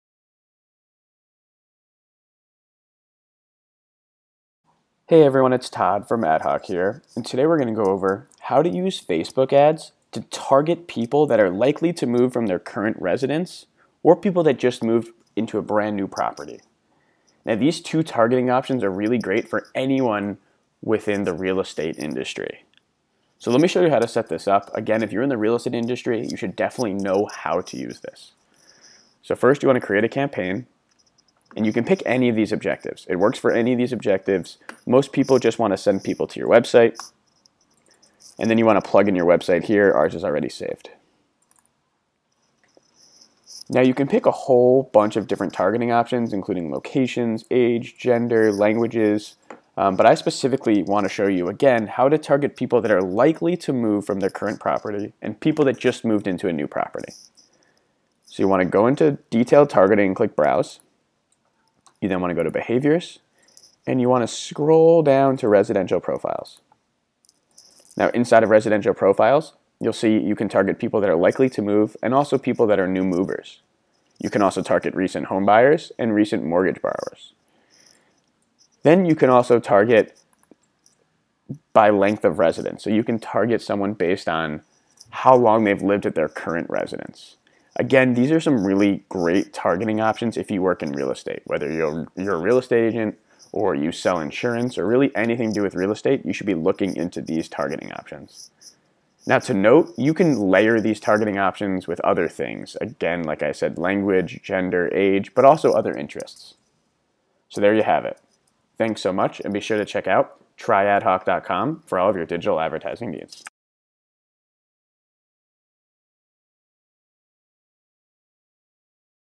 Facebook Ads Tutorial for Real Estate Agents/Brokers
A Facebook ads tutorial for real estate agents & brokers. Learn how to use Facebook ads to target people that are likely to move, or that just moved to a new home. and our blog if you want more tutorials related to digital marketing, PPC, Facebook ads, Google AdWords or Instagram ads.